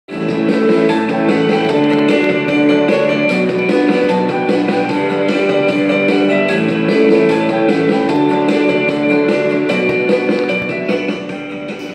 WIN WIN Android 11 Interactive Smart Board
65/75/86/98 inch Smart Interactive Flat Panel
Newest Android 11 Solution!!! Cheap price!
Widely used in Meeting Rooms and Classrooms.